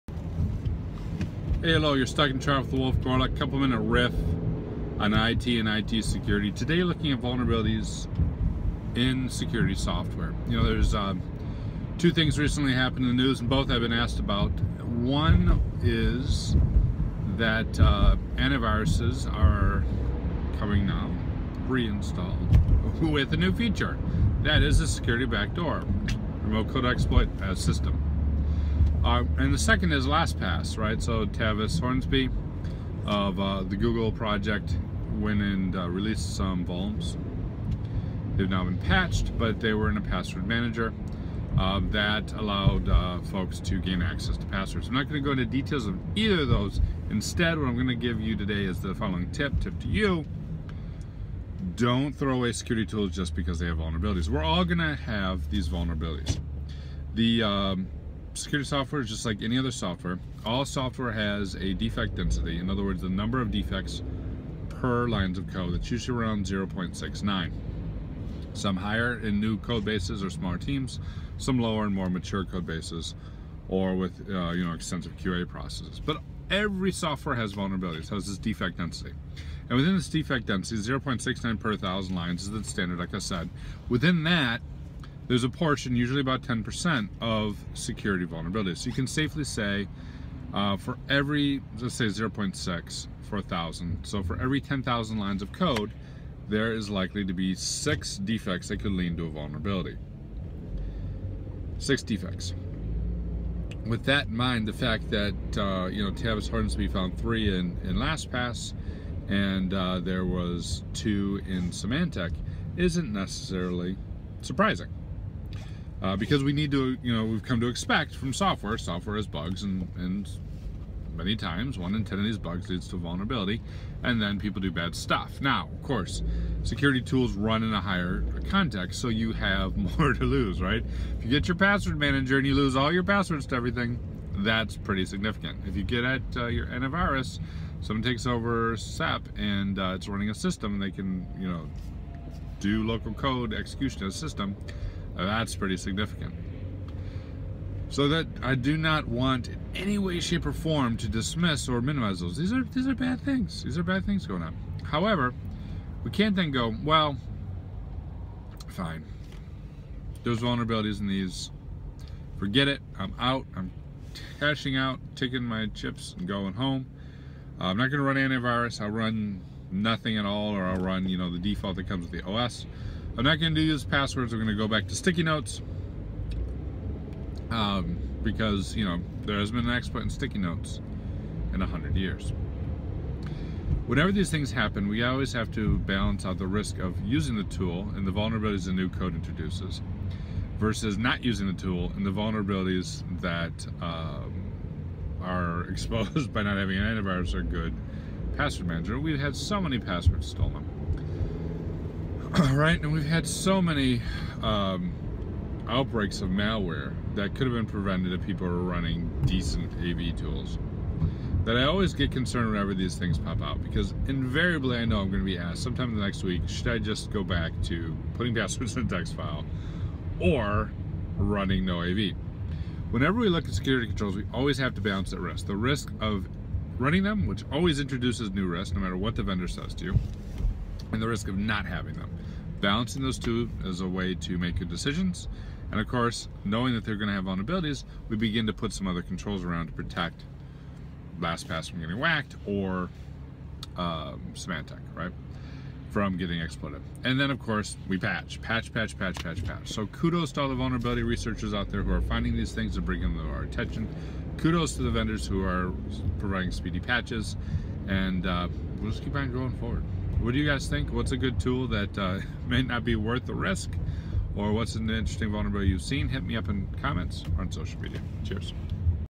Stuck in Traffic - Security Tools Exploited
At the same time frame, password managers and anti-malware tools get exploited. New vulnerabilities leads to the old question: is it worth running security tools?